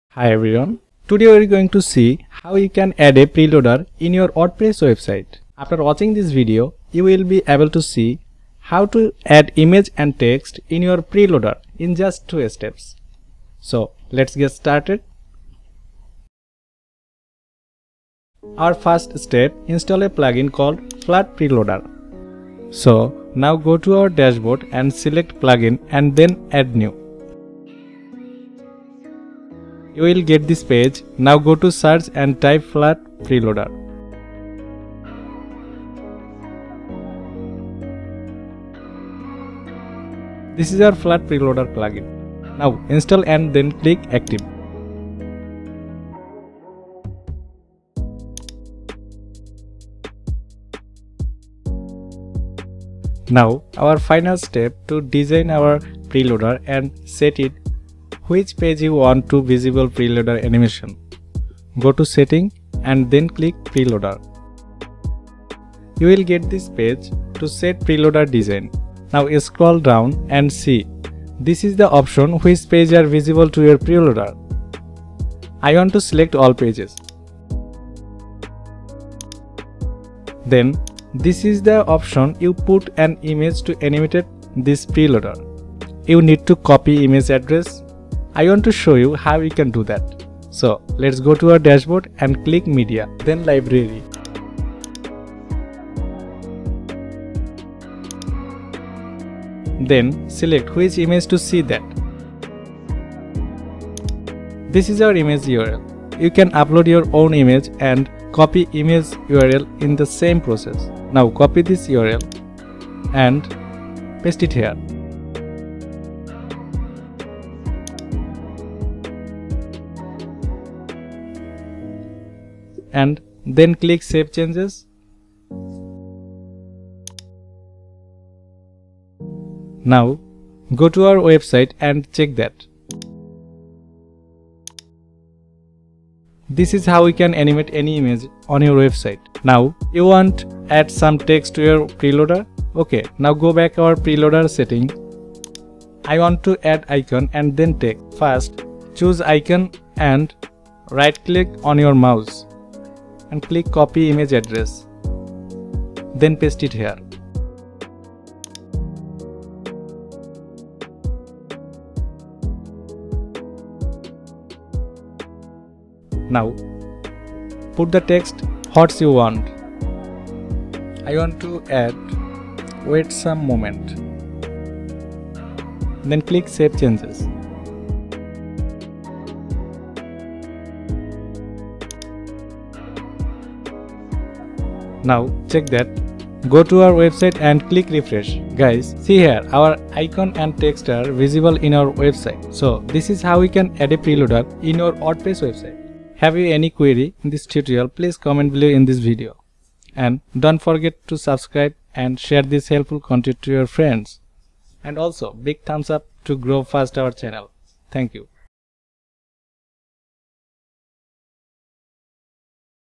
how to add a preloader in your wordpress website | DesignWithAI
Learn how to add a preloader in your Wordpress website?

After watching this video you will be able to create a image gallery on your website with elementor.

𝗧𝗮𝗯𝗹𝗲 𝗼𝗳 𝗖𝗼𝗻𝘁𝗲𝗻𝘁𝘀 :

00:00 Intro

00:25 install preloader plugin ( Flat Preloader )

01:29 Copy image address ( animation )

02:20 add text to our preloader

You can do that in just 2 steps